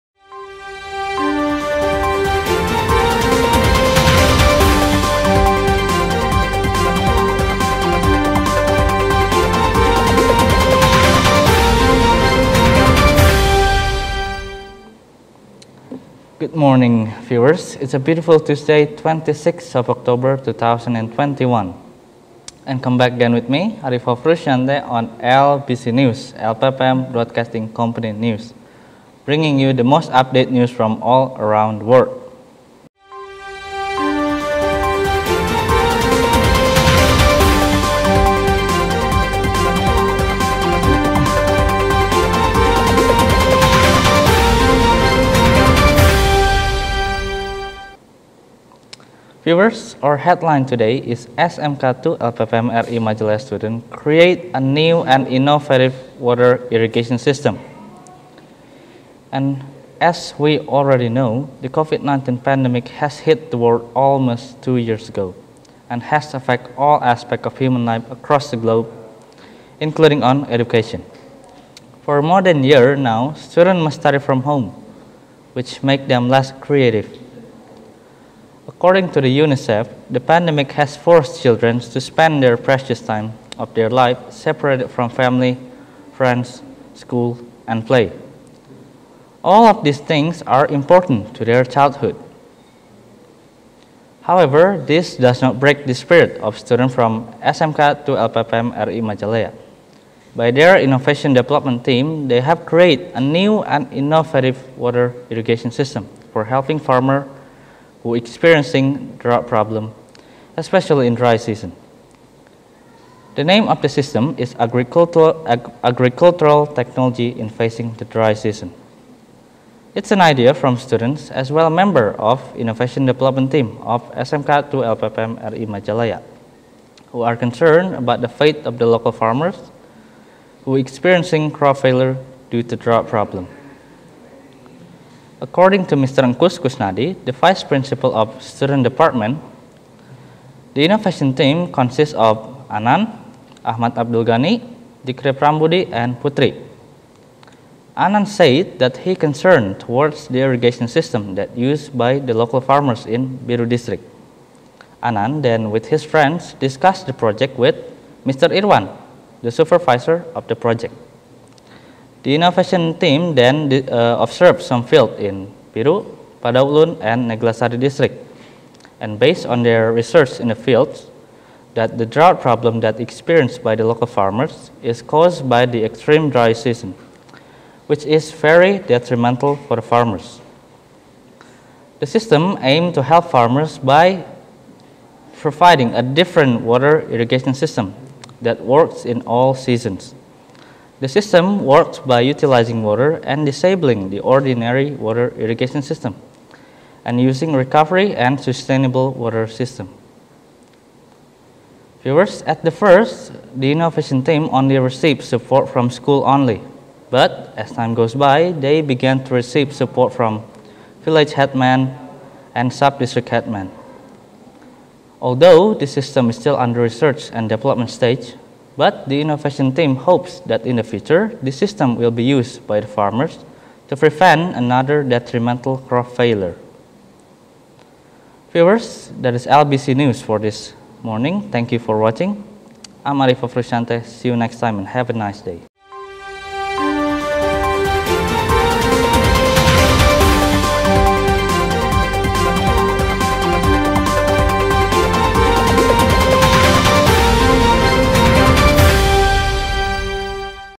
SMK 2 LPPM RI Majalaya Students Creates A new and Inovative water Irrigation system|Arrivo Fruciante
Siswa SMK 2 LPPM RI Majalaya Ciptakan Sistem Irigasi Air Baru dan Inovatif | Arrivo Fruciante
Produk Inovasi SMK 2 LPPM RI MAJALAYA